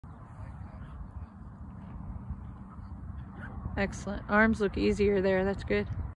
Swing Circle
Returning the club from the circle - forward swing. Gaining a vision and a feel.